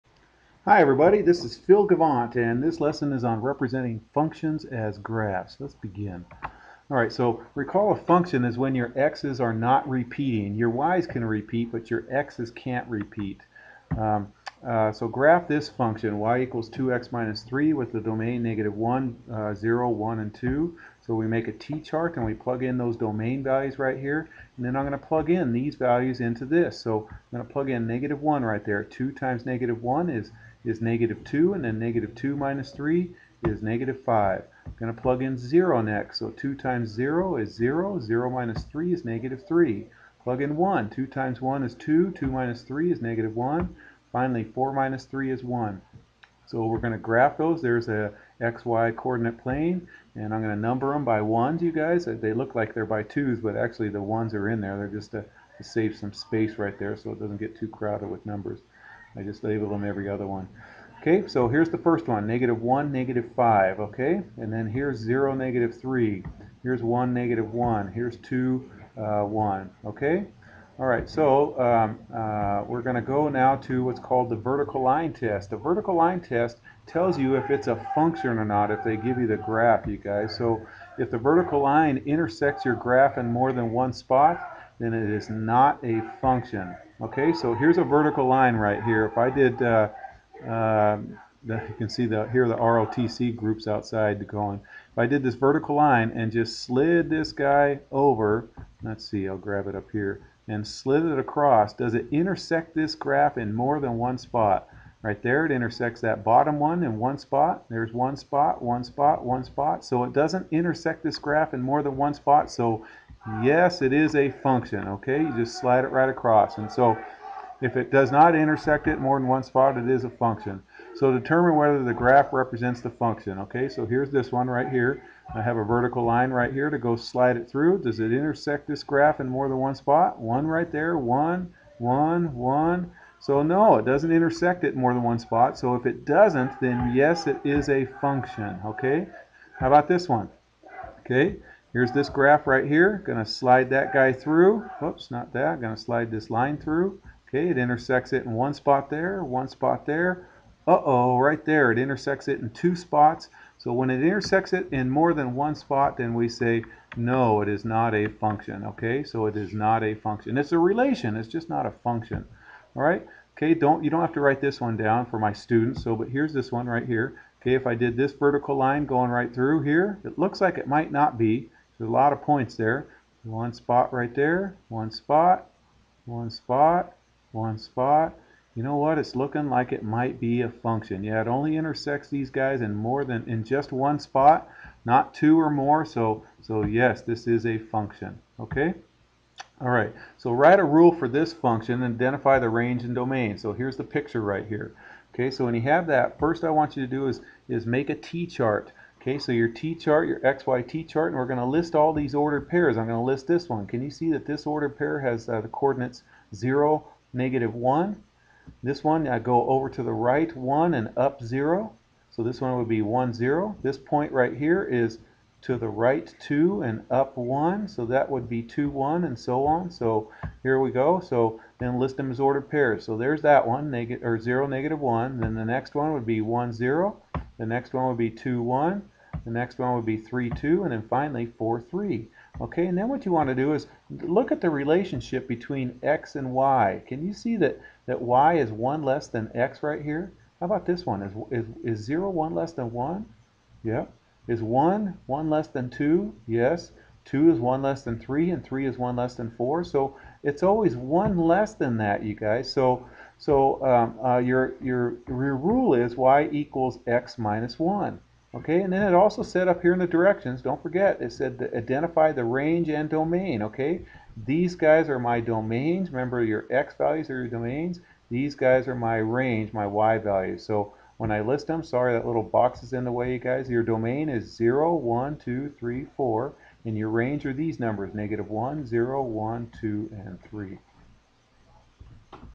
Representing Functions as Graphs
This Algebra 1 lesson shows you how to graph functions and use "The Vertical Line Test." It's a must see if you're interested in a relationship!